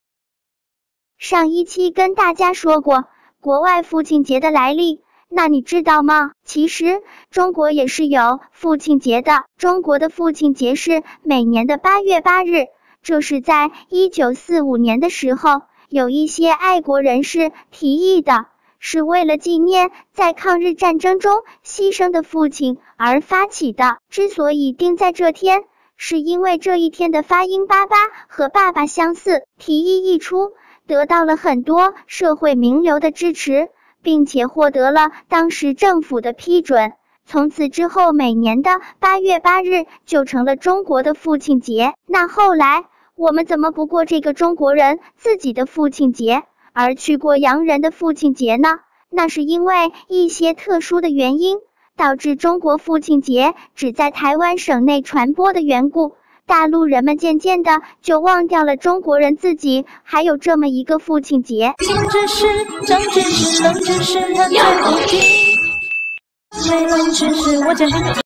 歪歌社团 十万部冷知识：中国的“父亲节”为什么在8月？
上一期跟大家说过，国外父亲节的来历，那你知道吗？其实，中国也是有“父亲节”的……